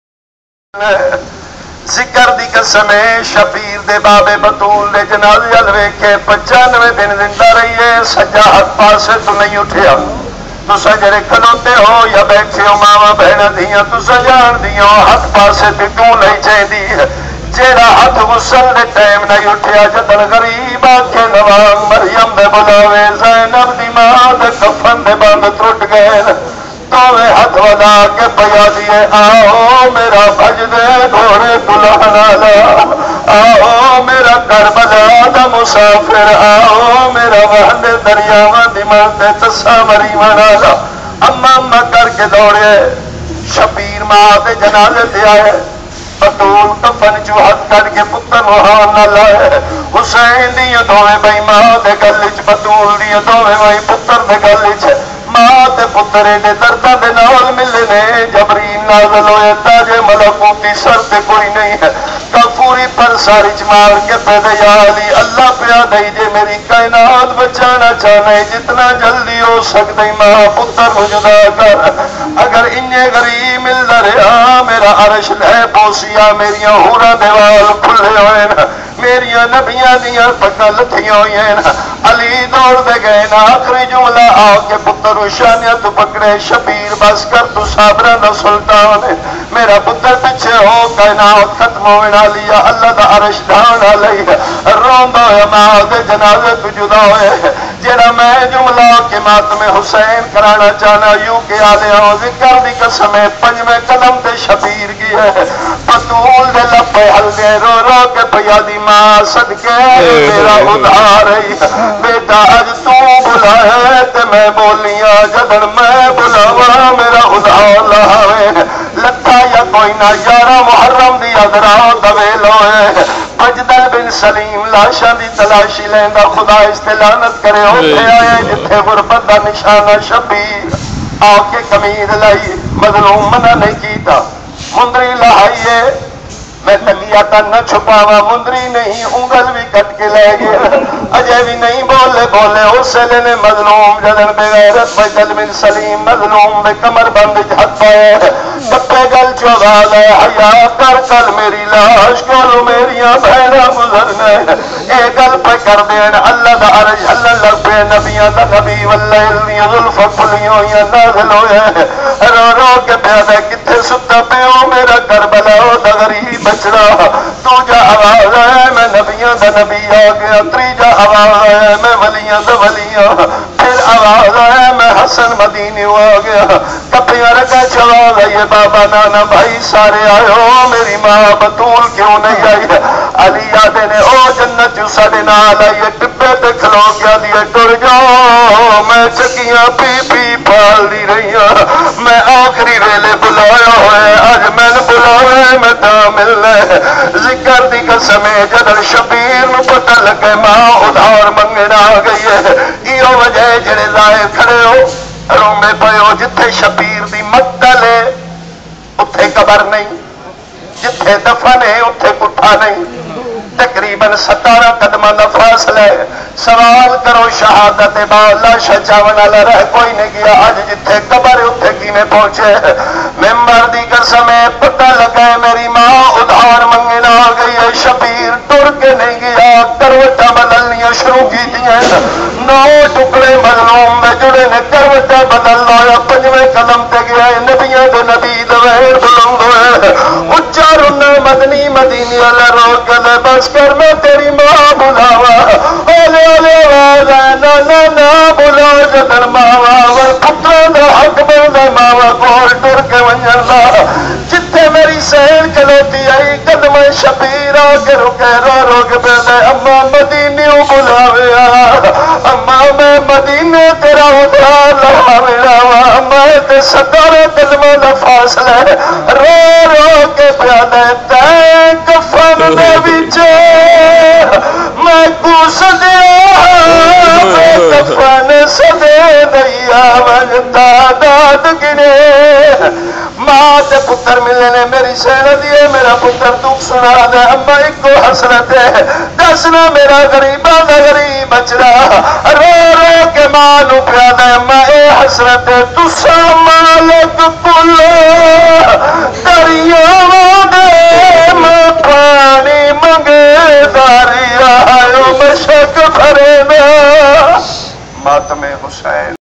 Masaib bibi Fatima(a.s) by Iqbal Shah Bajar outside SAUDI Embassy LONDON 2011
Outside saudi embassy london. Masaib bibi Fatima(a.s) recited by Syed Iqbal Shah Bajar of Pakistan. Curse of Allah upon All e Saud and Bani umayah, protest was organised bu Tehreek e nifaaz e Fiqhe Jafaria UK and AGHA Northampton